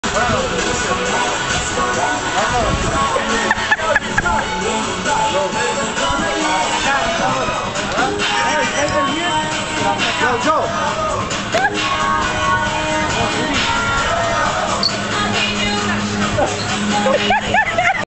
my brother being a retart
My brother dancing with some lady at Wildwood NJ.